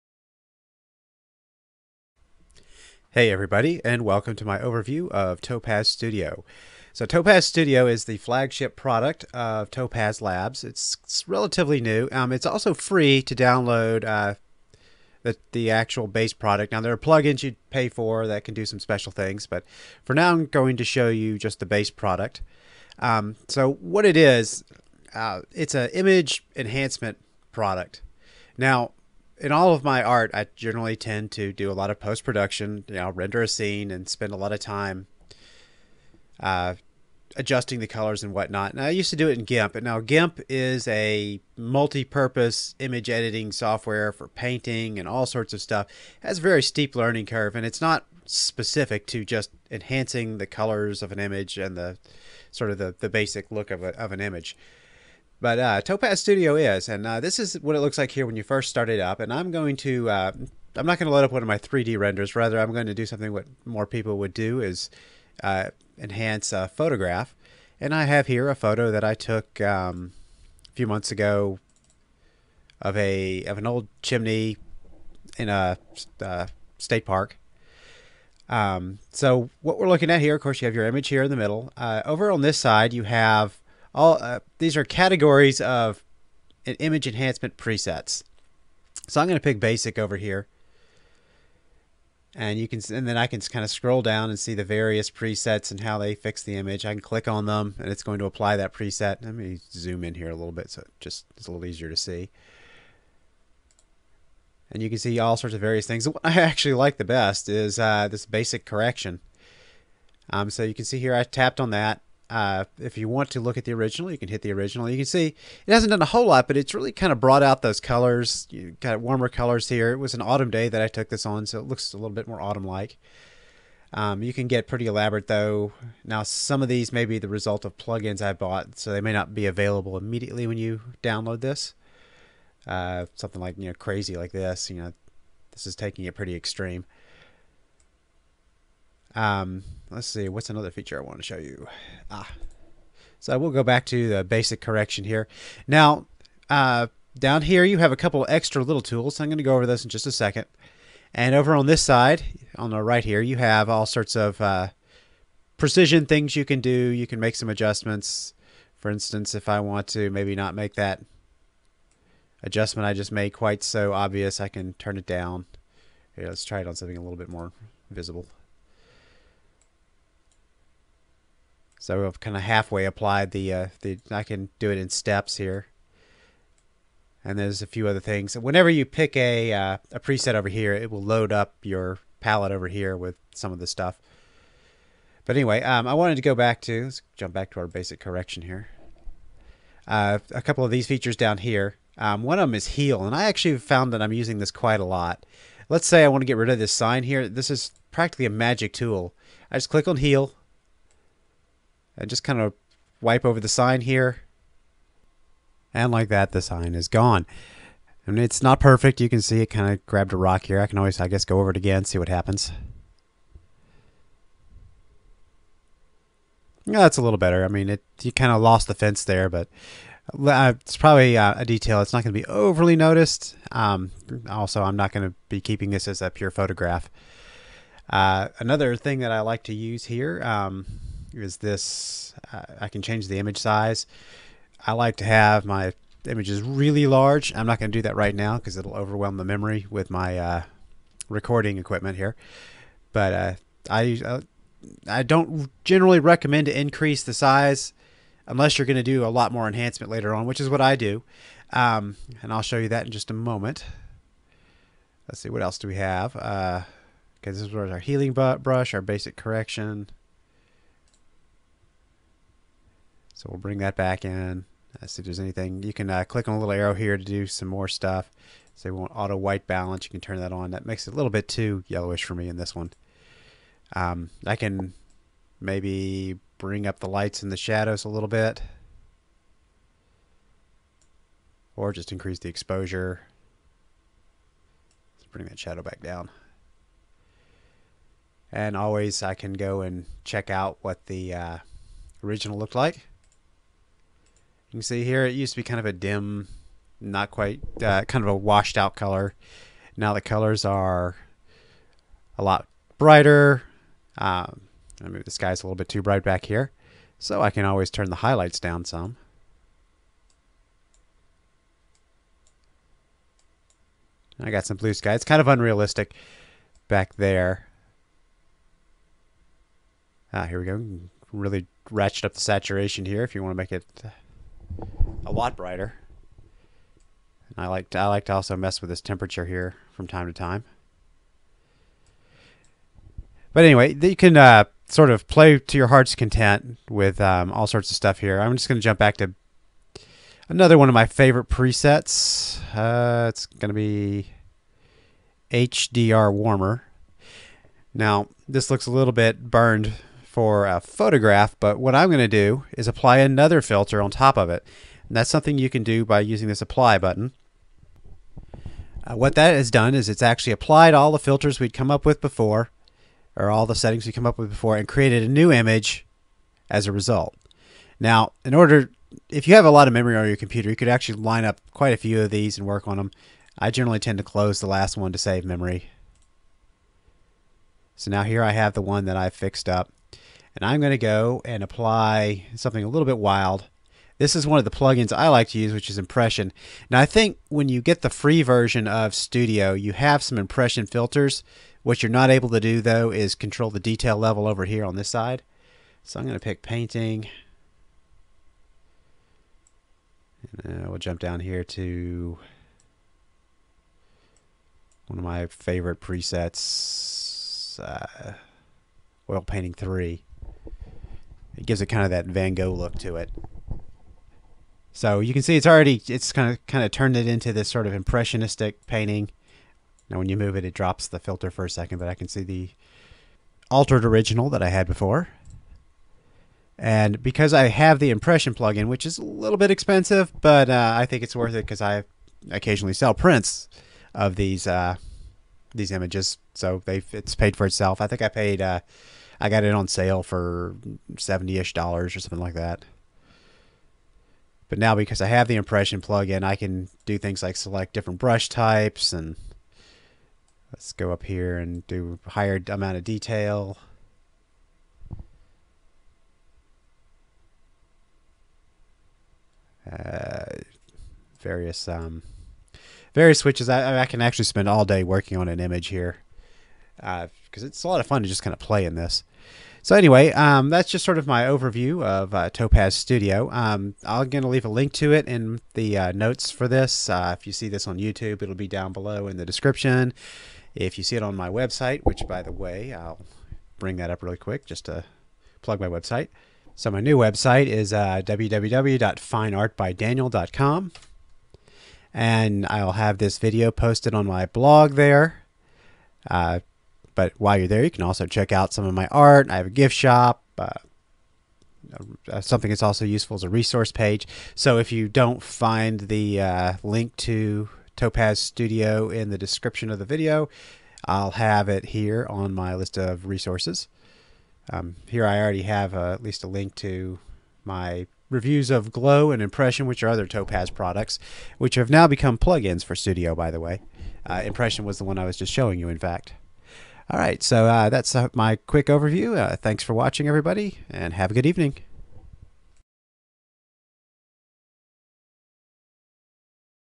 A Quick Overview of Topaz Studio
Topaz Studio is the flagship product from Topaz Labs.  I've always like Topaz products and Studio is no exception.  It's single job is to enhance digital images, and it does a great job at it.

The majority of my art, I create digitally using 3D rendering algorithms and digital painting programs.  For almost all of my art, I have a post production phase where I adjust the final images, working the color balance, contrast, saturation, etc.  For years, I used GIMP for this.  GIMP however is a fully featured image editing program that allows everything from painting to tweaking colors.  It does a bit of everything, but it's not a specialist.  Plus, it's got a steep learning curve.

Later on, I discovered Topaz Adjust which was a tool specifically for enhancing images.  It was a plugin for Photoshop and did okay.  Rather than spending several hours in GIMP in post production, I could spend about half the time in Topaz adjust.  There results were pretty good, but not great.

Now there is Topaz Studio.  I recently downloaded this stand-alone application to give it a try and I LOVE it.  It's got a lot more tools than Adjust, is easier to use, and seems to handle memory better, being more responsive and less crashy.  Quite often just a single click on the "Basic Correction" gives me the perfect results.

Topaz Studio is FREE.  You have to create an account on the Topaz site to download it, but that's it for most of the foundation level image filters.  You can however add plugins to Studio.  Many of Topaz's products have been converted into plugins.  For instance, I almost immediately picked up the new plugins for Topaz Impression and Topaz Glow, two applications that have always impressed me.  The plugins might cost a bit, but for someone like me who sells prints of art created using Impression and Glow, I think they are well worth the cost.